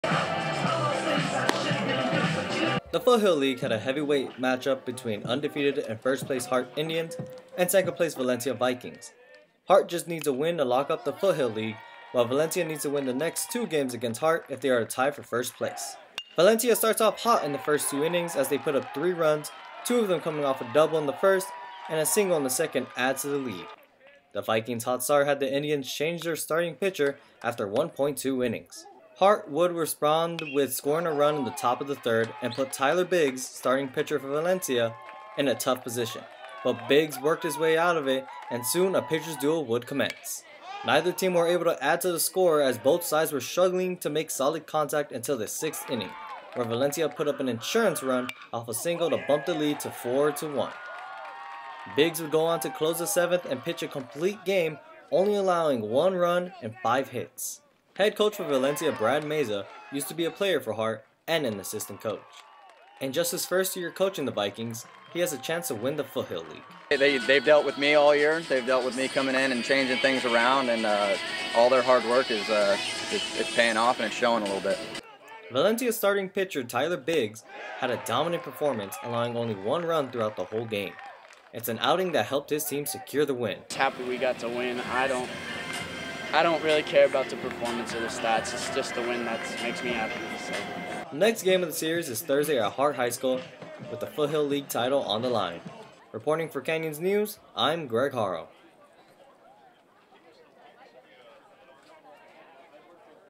Undefeated baseball team Hart High takes on a strong Valencia team
Hart High baseball looked to remain undefeated in league play yesterday as they took on second place Valencia.
Greg Haro was there for the Foothill league showdown.